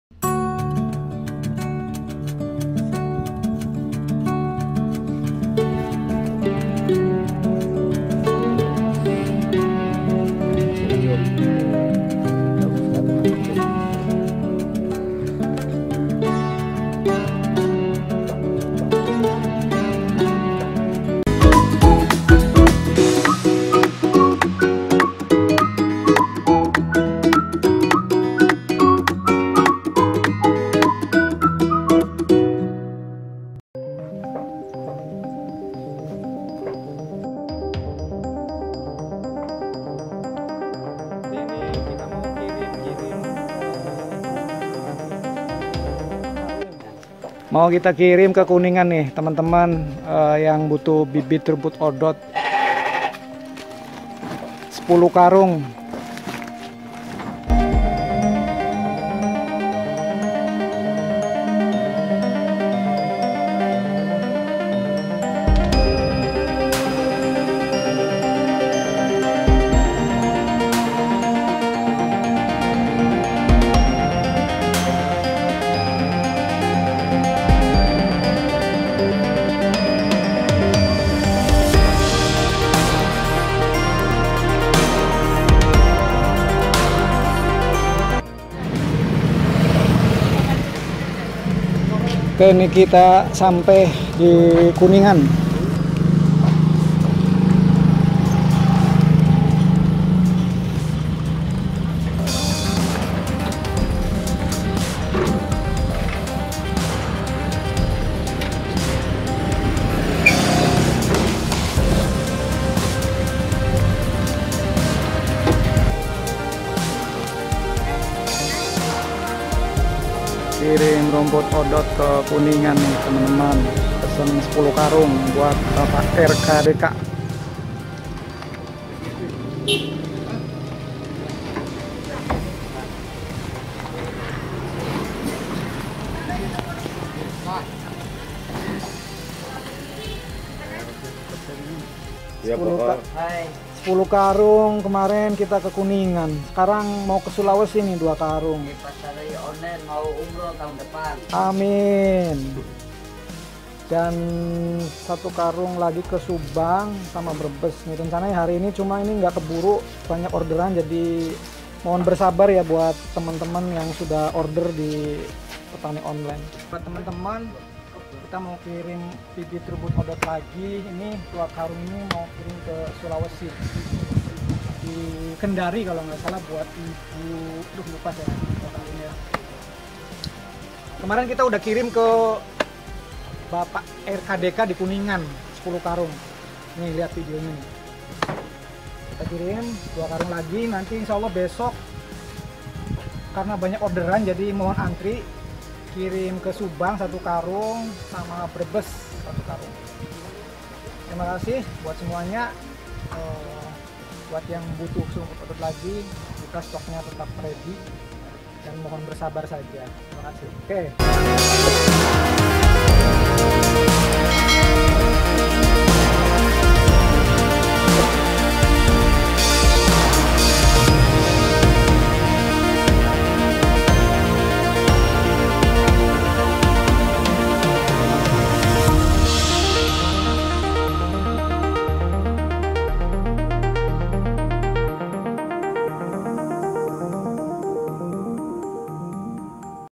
PENGIRIMAN BIBIT RUMPUT ODOT Ke KUNINGAN JAWA BARAT dan Ke MAROS SULAWESI SELATAN
Tetap setia melayani para pemesan bibit rumput odot. Kali ini kita mau kirim kirim bibit Odot ke Kabupaten Kuningan Jawa Barat dan ke Kabupaten Maros Sulawesi Selatan.
Bagi yang membutuhkan bibit Odot silahkan hubungi kami di WA : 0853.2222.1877
*kirimbibitodot